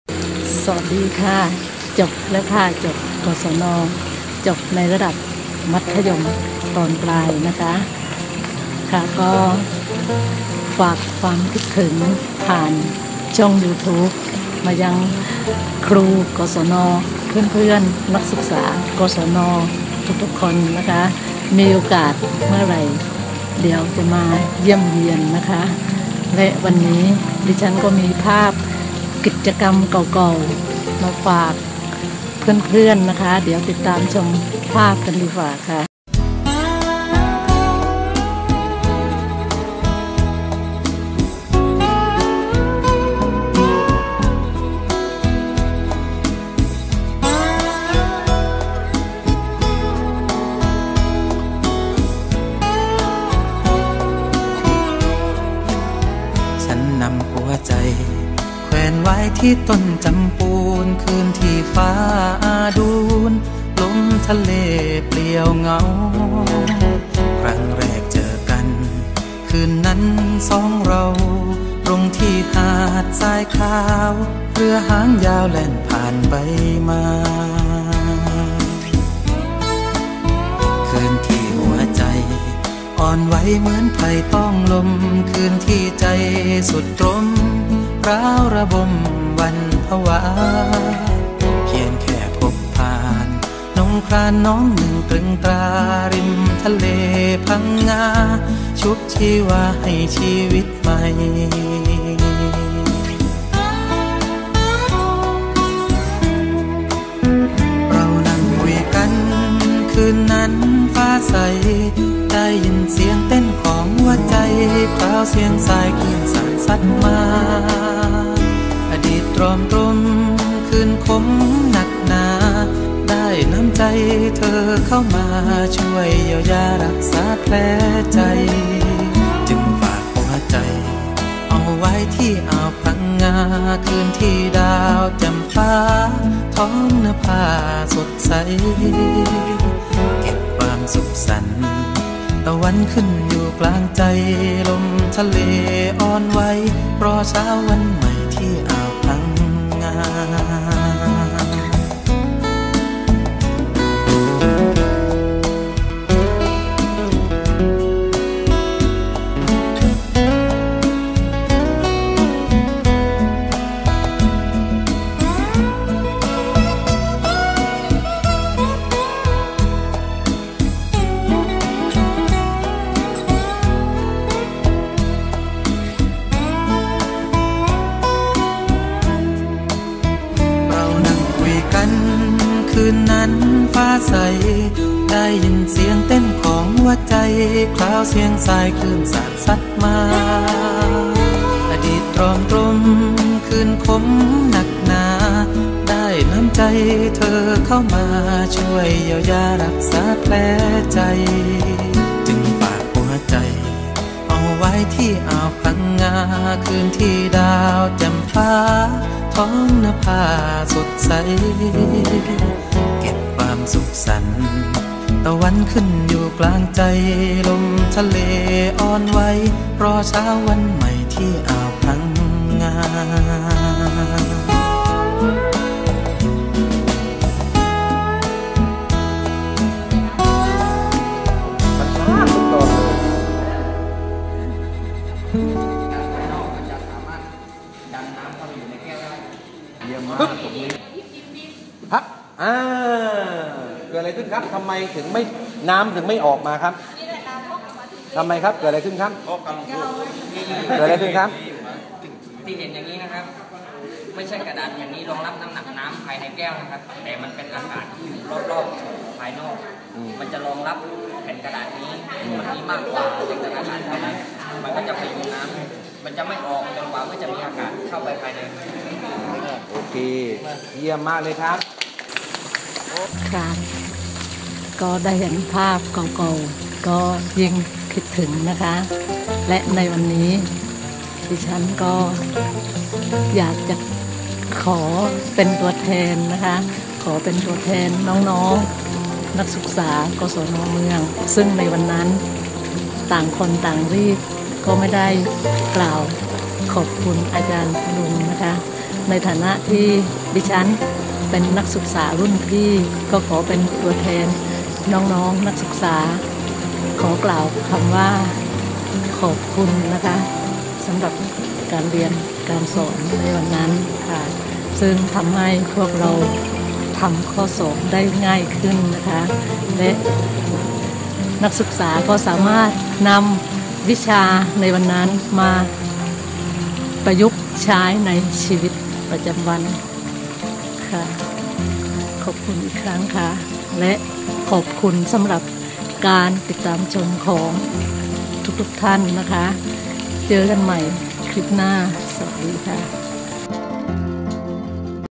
คิดถึง กศน.สถาบันการศึกษาที่เปิดโอกาสให้ทุกชนชั้นได้เข้ามาเรียนรู้
คิดถึง กศน.สถาบันการศึกษาที่เปิดโอกาสให้ทุกชนชั้นได้เข้ามาเรียนรู้ เพื่อยกระดับความรู้และวิทยะฐานะให้สูงขึ้น